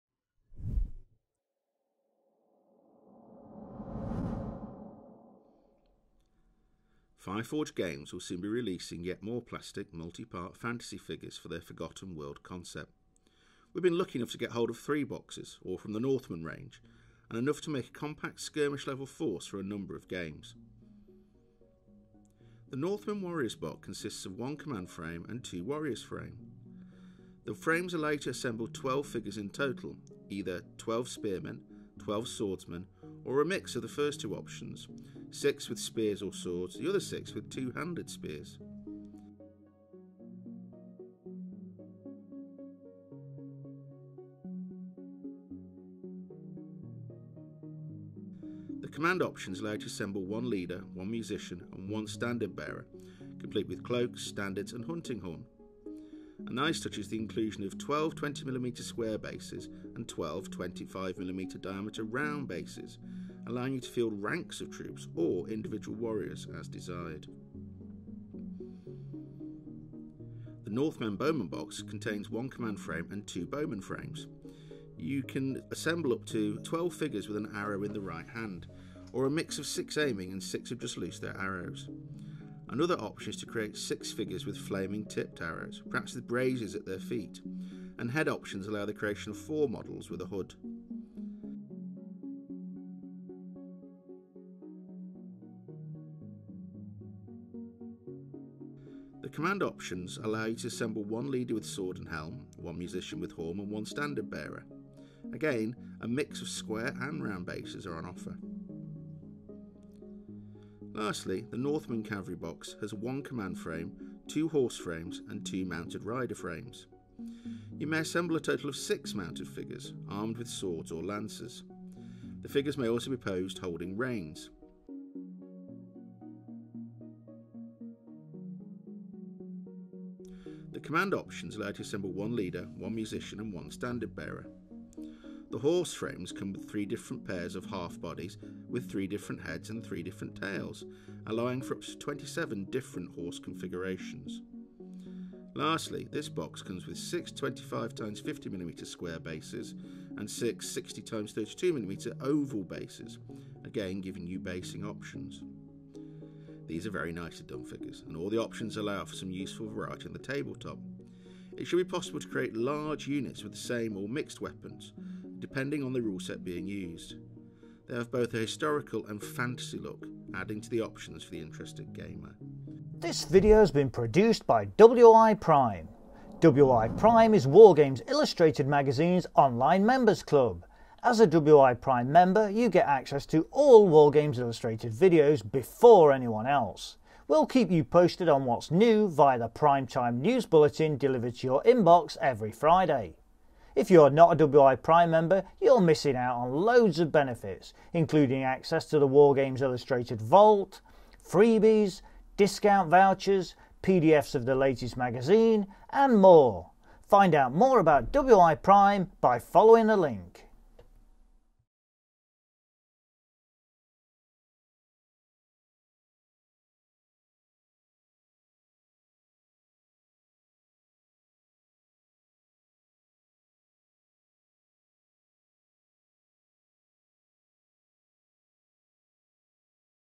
Forgotten World Plastic Northmen Miniatures | Fireforge Games | Unboxing
Fireforge Games will soon be releasing yet more plastic, multi-part fantasy figures for their Forgotten World concept. We’ve been lucky enough to get hold of 3 boxes, all from the Northmen range, and enough to make a compact skirmish level force for a number of games.